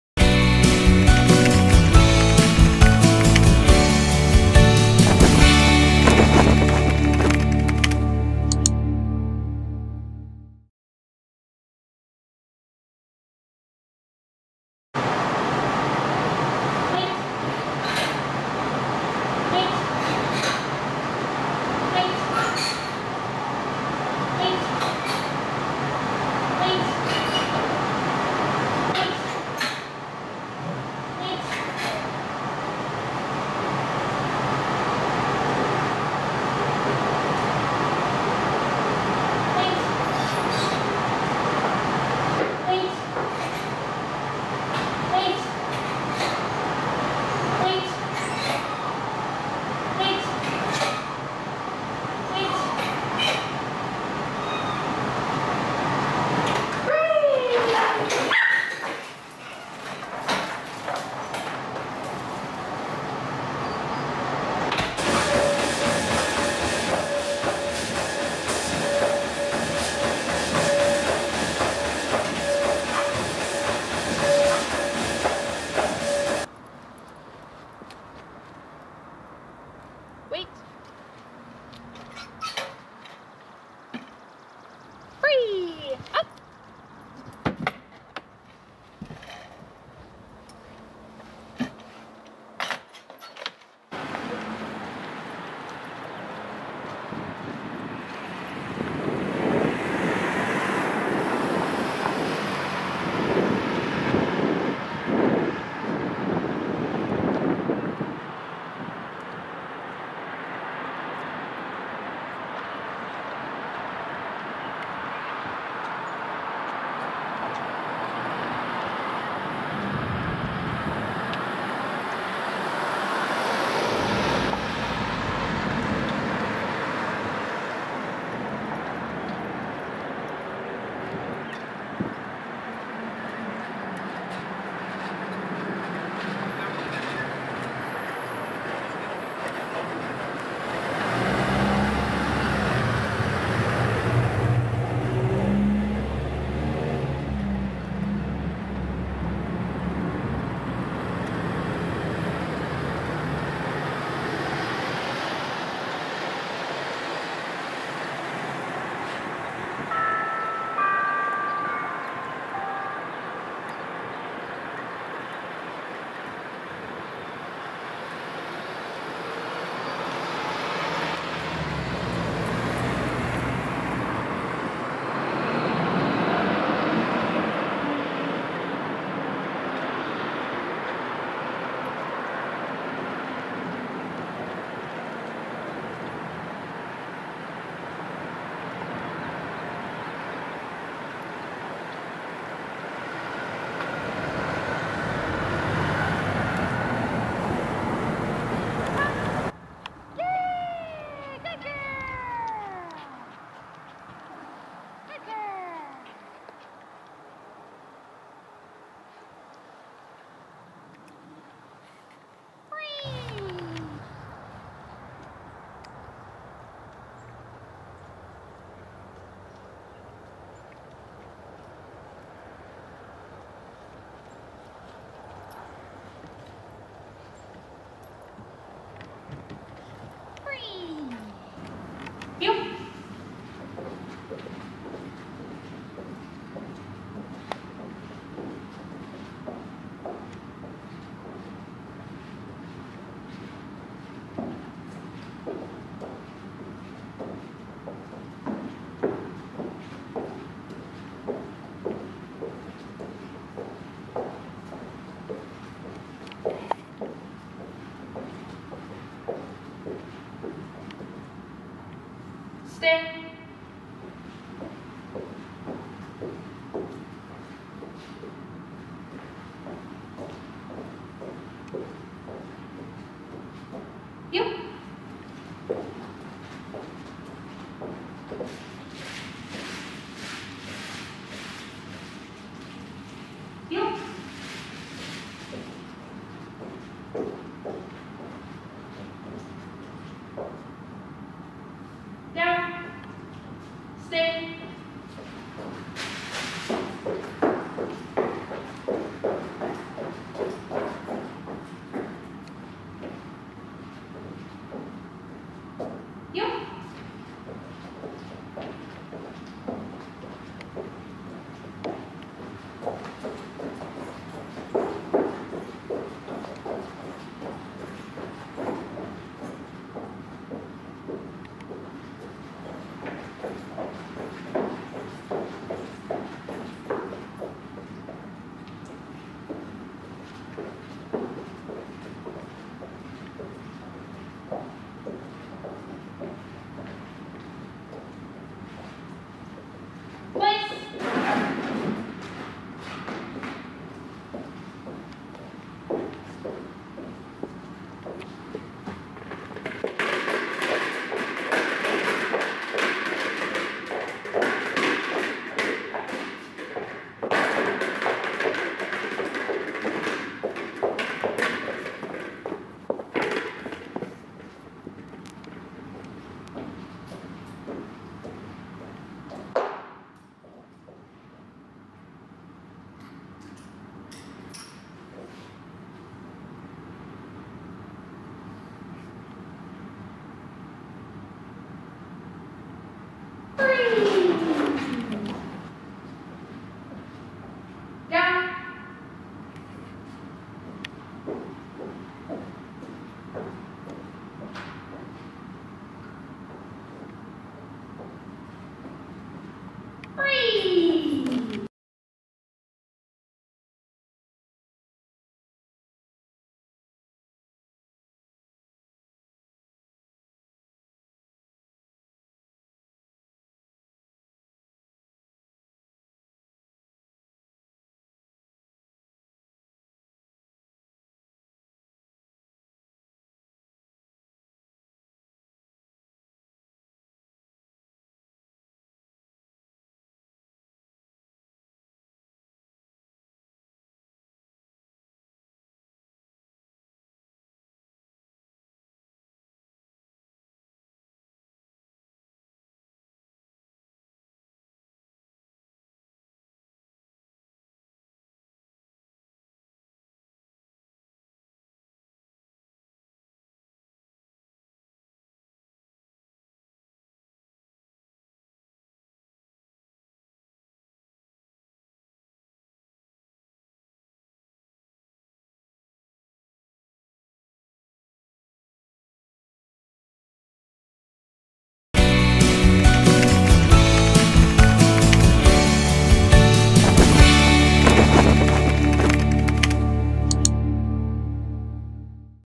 Harley (Labrador Retriever) Puppy Camp Dog Training Video Demonstration
Harley (Labrador Retriever) graduated from the dog training puppy camp at Neuman K-9 Academy. This program included obedience commands to sit, stay, heel or walk on a loose leash, come when called, proper etiquette, no jumping up, meeting and greeting people under control, and running on a treadmill.

Our dog training camp provides programs for Labrador Retriever's such as boot camp, obedience training, and puppy camp.